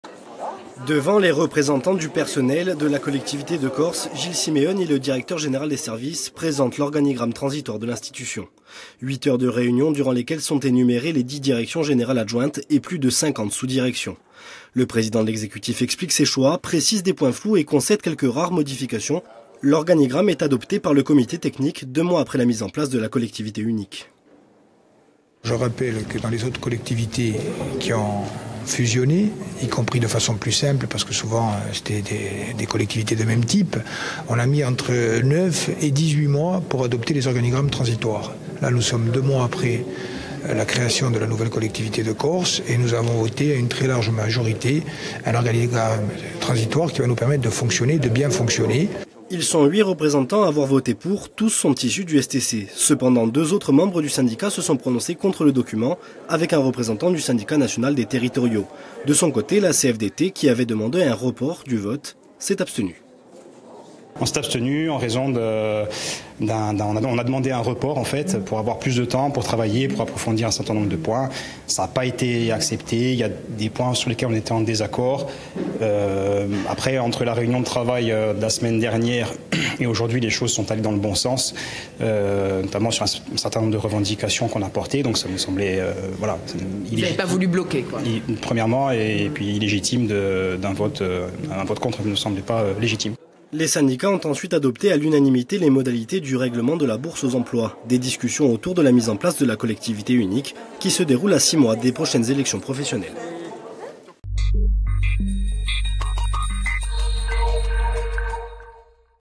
L’organigramme transitoire de la nouvelle collectivité de Corse adopté
Intervenants : GIlles Simeoni, président de l'exécutif de Corse.

Reportage : SIMONPOLI Pierre ; LEONI Delphine ; AGOSTINI Stéphane.